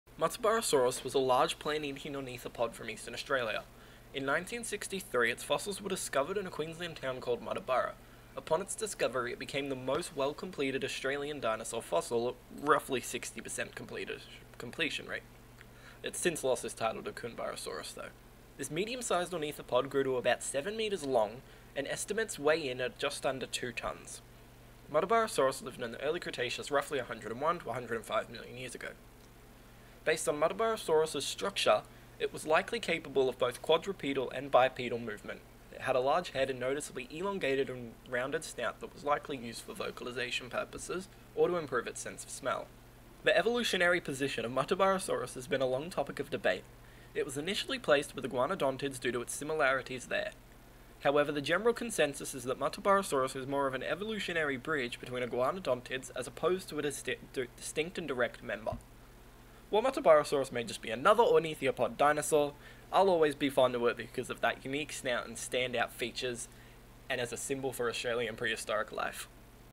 Learn About MUTTABURRASAURUS!!
Learn about this magnificent Aussie dinosaur. Muttaburrasaurus.

References.
**NONE OF THESE IMAGES ARE MY OWN**